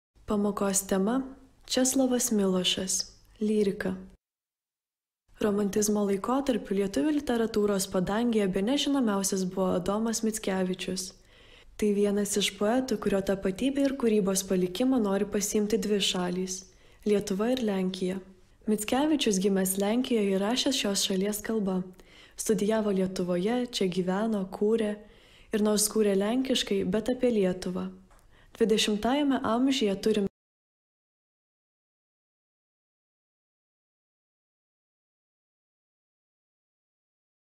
Česlovas Milošas – Lyrika - Mokslinčius.lt - Tavo mokytojas telefone !
Šią pamoką skirsime Česlovui Milošui – pasaulyje žinomam lenkų poetui, gyvenusiam Lietuvoje. Pradžioje susipažinsime su svarbiausiomis rašytojo gyvenimo detalėmis, o vėliau pakalbėsime apie kūrybos ypatumus. Sužinosite, kokie motyvai dažnai sutinkami Č. Milošo poezijoje. Plačiau išanalizuosime gimtojo krašto vardu pavadintą eilėraštį “Šeteniuose”.